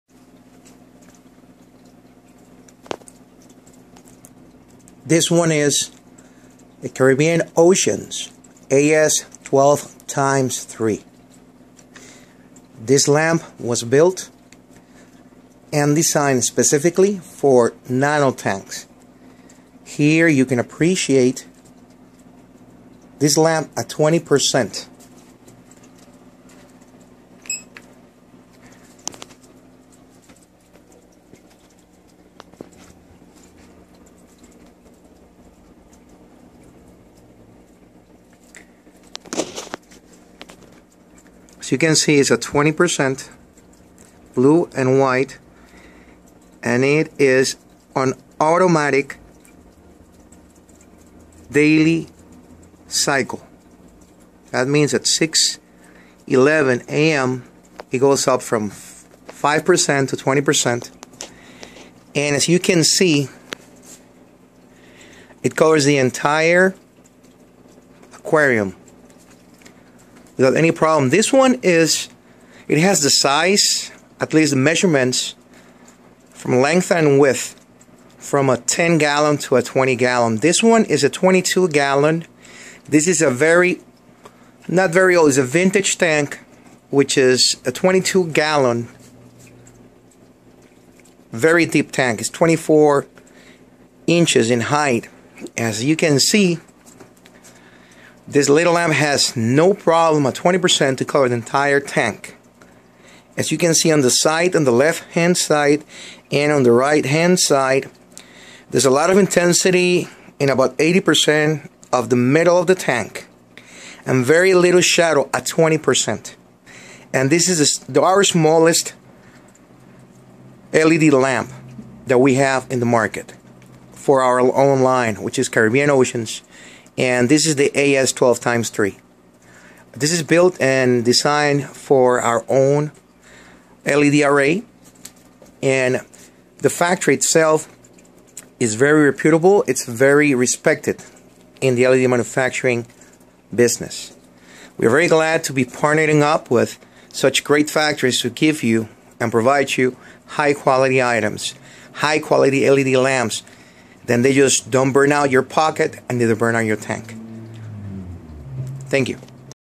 Caribbean Ocean's Nano Reef led full spectrum lamp, the AS-12×3 in action!
Just an example of the power built-in of our Nano Reef Led lamps. Here you can appreciate the full spectrum coverage of our AS-12×3W applied to a Nano column tank that is 24" in depth. The colors are amazing and the penetration of the light is stunning.